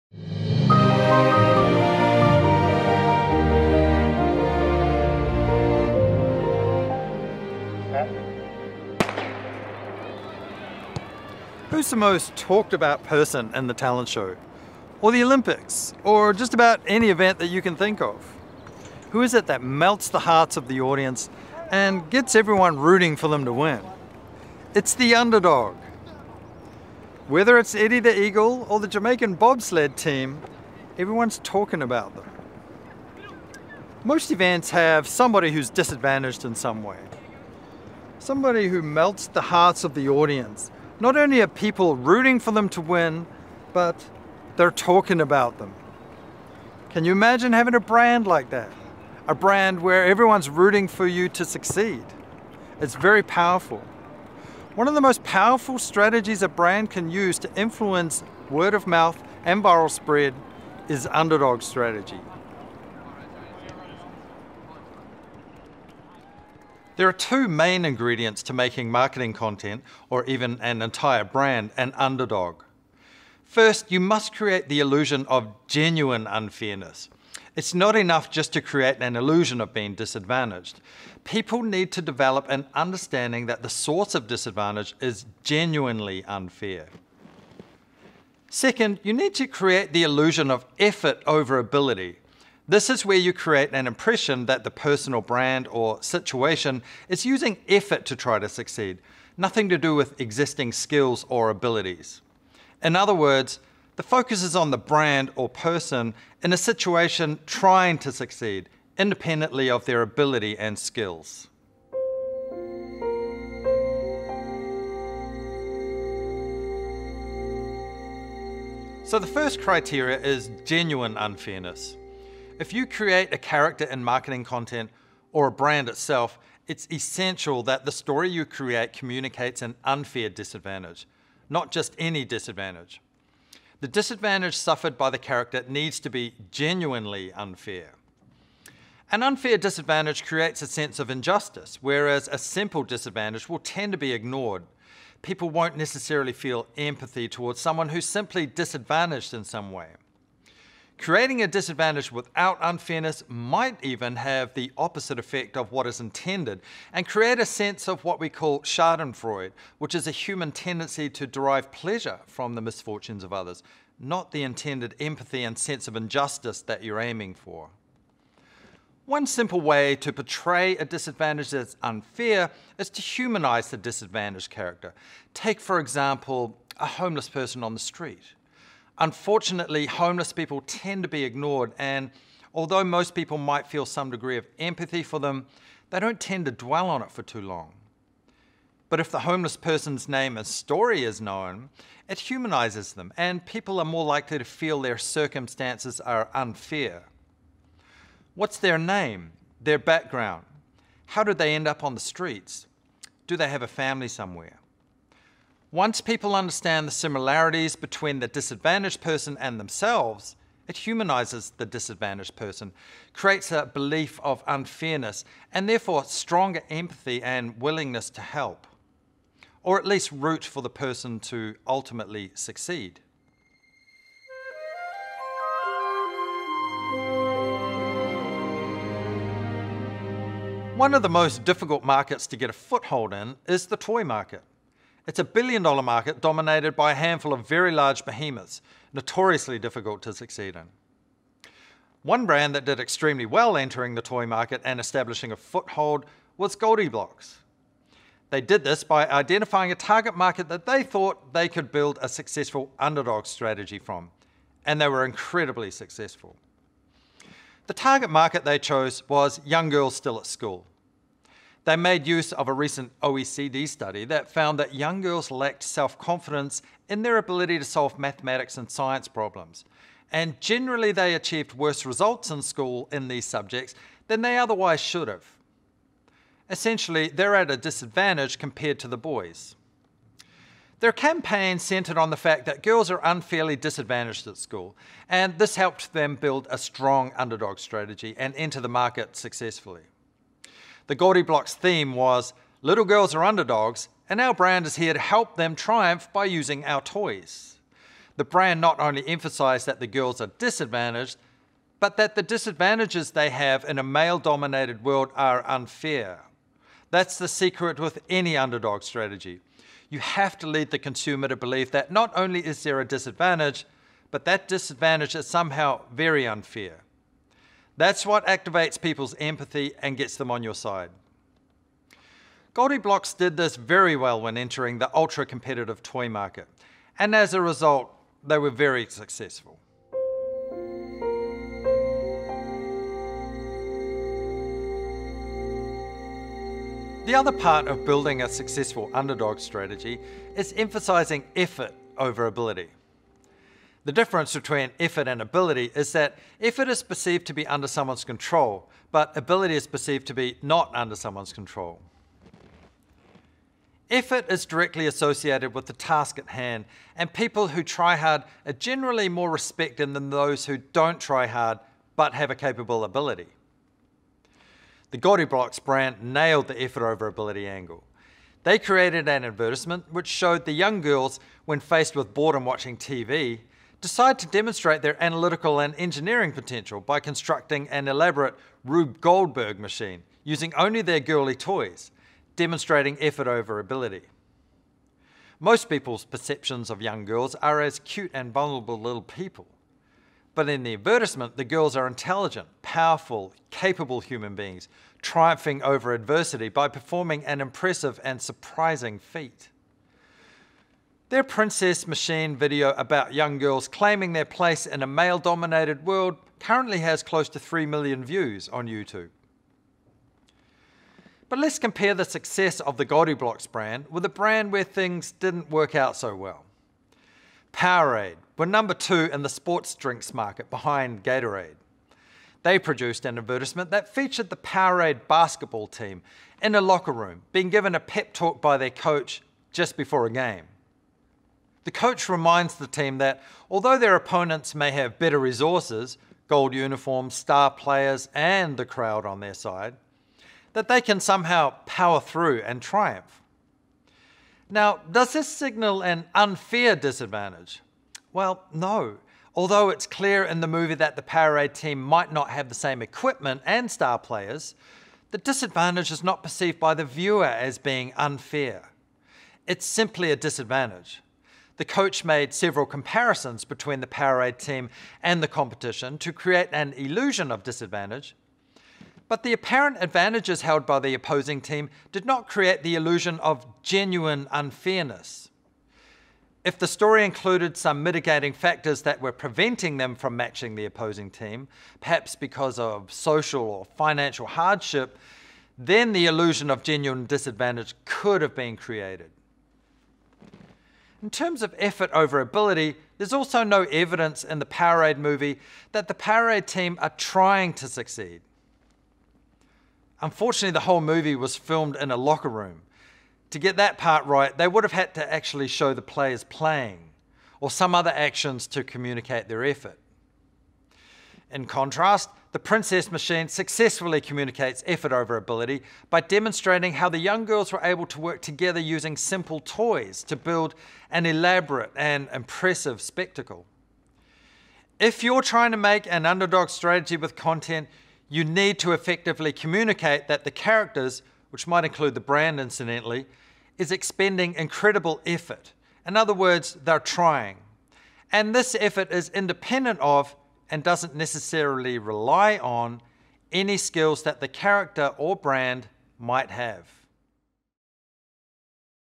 VIROLOGY: The science of Viral Marketing. Chapter 7 Underdog strategy
CHAPTER 7: UNDERDOG STRATEGY
One of the most powerful strategies a brand can use to create word of mouth and viral spread is underdog strategy. Brent shares how to make one.

BRENT COKER'S VIROLOGY MASTERCLASS

What makes content, or a brand or idea, go viral?

In this Masterclass you’ll learn the science behind why things go viral, the psychology of building a strong brand, and how to create messages that people can’t resist sharing.